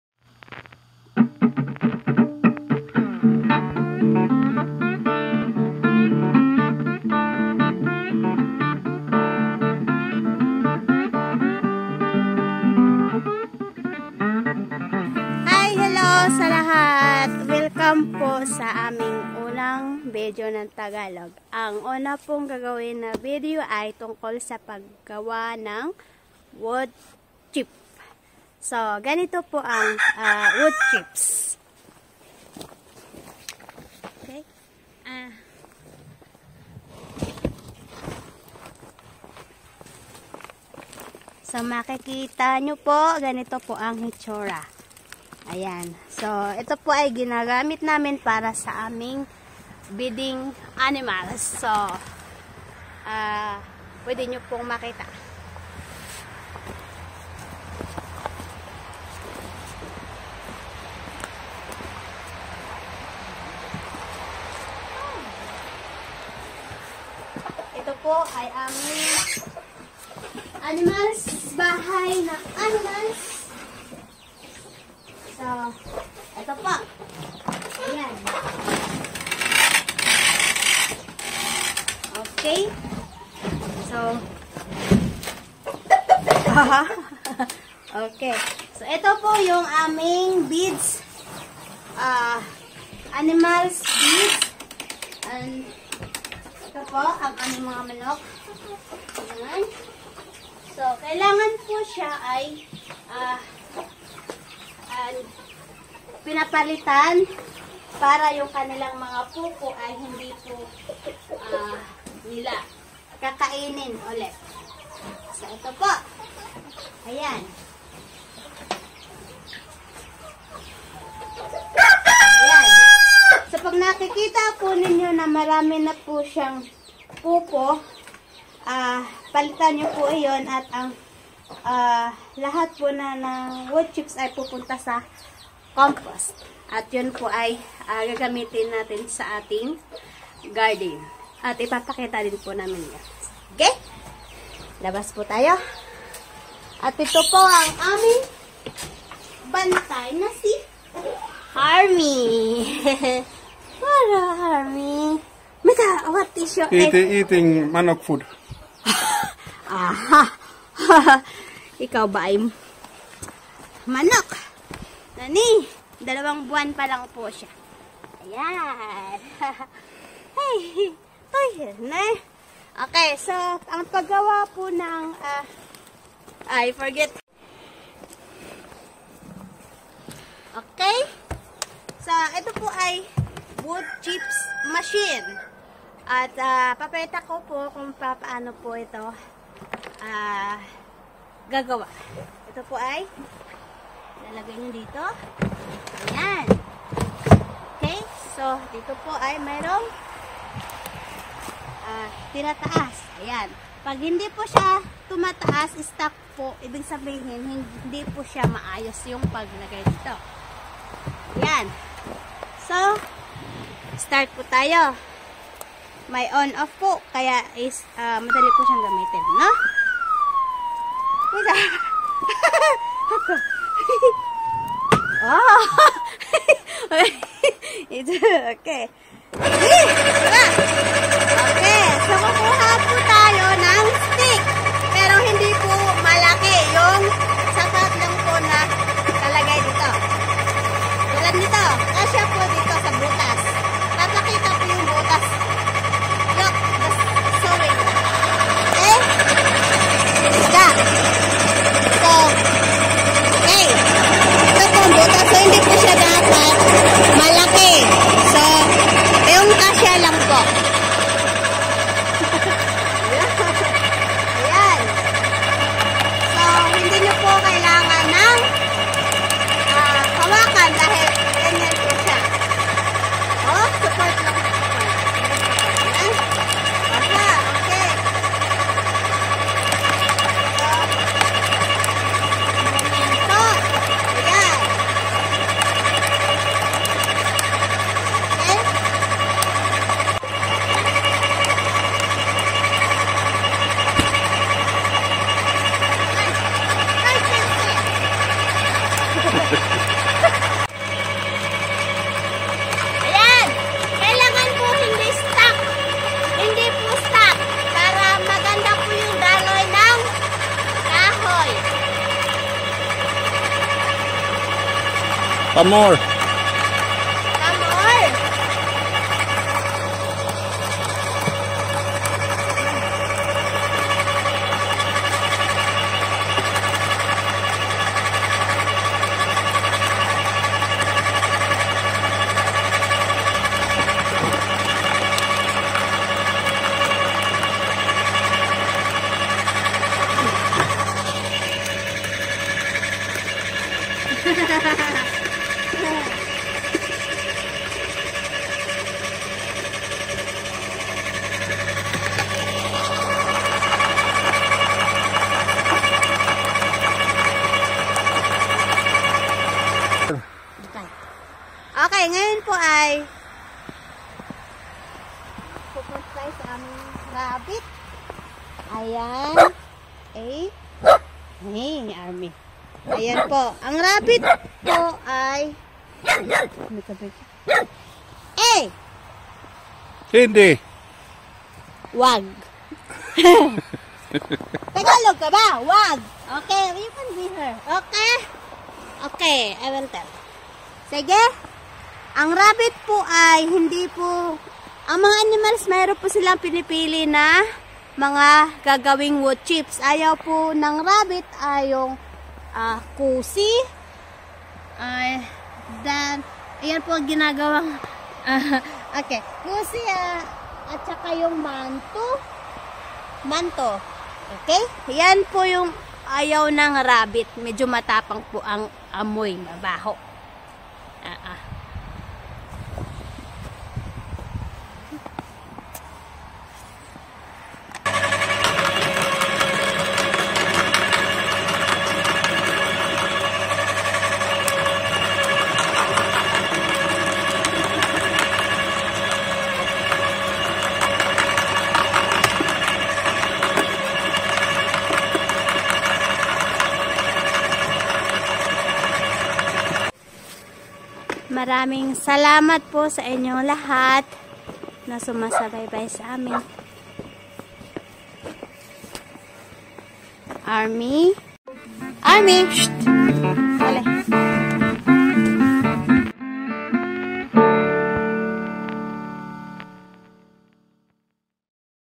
Paano gumawa ng woodchips
woodchips ginagamit namin para sa bedding animals at garden

Lifestyle
Organic Farming
Simple life
Permaculture